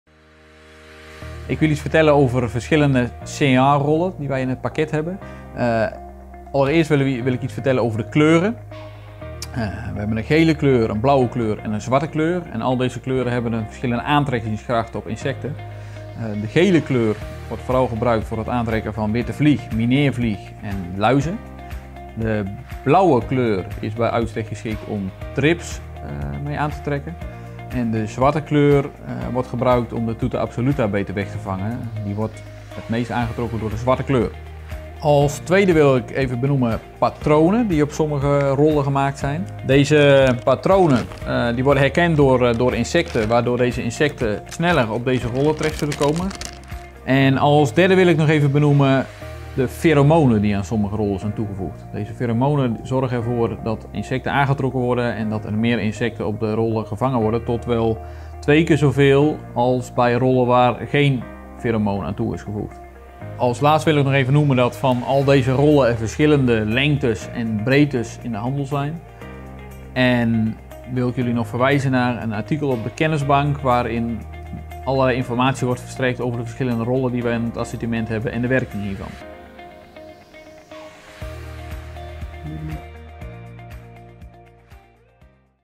Welke soorten signaalrollen zijn er?
Het tijdig signaleren van plagen in een gewas is van groot belang, want hoe eerder een plaag gesignaleerd wordt, hoe eerder je kan starten met het bestrijden van de plaag. Voor het signaleren van een plaag kan je onder andere gebruik maken van signaalrollen. In deze video laat onze specialist Henk van Daalen zien welke verschillende soorten signaalrollen er zijn en licht hij toe wanneer je wel type signaalrol het beste kan inzetten. Het vanglint wordt boven het gewas gespannen om plagen weg te vangen. Naast de verschillende kleuren lint, zijn de rollen ook verkrijgbaar met patroon of met patroon en feromoon.

Gele signaalrol
De gele signaalrollen hebben een grote aantrekkingskracht op hoofdzakelijk witte vliegen, maar ook op bladluizen, mineervliegen, tripsen en varenrouwmuggen. De signaalrollen zijn gemaakt van een hoge dichtheid polyethyleen en kennen een lange levensduur. De signaalrollen zijn aan beide kanten voorzien van een krachtige lijmlaag. De kwaliteit van de lijm is dusdanig dat deze ook na een beregening en/of verneveling nog steeds plakt. Het doel is te allen tijde het wegvangen van zoveel mogelijk insecten. Uit onderzoek is gebleken dat de signaalrollen het gehele teeltseizoen blijven hangen.

Blauwe signaalrol
Blauwe signaalrollen met feromoon zijn speciaal ontwikkeld om californische trips weg te vangen. Uit onderzoek blijkt dat de combinatie lokstof en het gedrukte patroon extra aantrekkingskracht heeft op zowel mannelijke als vrouwelijk californische trips. Dit maakt het dat deze signaalrollen telers een extra hulpmiddel toereikt voor de signalering en bestrijding van californische trips. De signaalrollen zijn te gebruiken in zowel groenten als sierteeltgewassen. De signaalrollen zijn aan beide zijden voorzien een krachtige en gebruiksvriendelijke lijm. Deze lijmsoort plakt niet aan de vingers, maar zorgt er wel voor dat het de gevangen insecten zeer goed vasthoudt. Geadviseerd wordt om de signaalrollen in het gewas te hangen, zodra het poten van de planten plaatsvindt. De signaalrollen kunnen worden ingezet in combinatie met biologische gewasbescherming en kunnen bijdragen aan het verbeteren van het bestrijden van plagen.

Zwarte signaalrol
De zwarte signaalrollen met feromoon (Tuta Black) zijn speciaal ontwikkeld om Tuta absoluta (tomaatmineermot) weg te vangen. De lokstof is verwerkt in de lijm en zorgt voor een constante en langdurige aantrekkingskracht. Deze signaalrol is een hulpmiddel voor de bestrijding van Tuta absoluta en plakt niet aan de vingers, maar zorgt er wel voor dat het de gevangen insecten zeer goed vasthoudt.

Meer weten over signaalrollen? Lees dan ons Kennisbankartikel met tips voor het signaleren van plagen

Direct signaalrollen bestellen?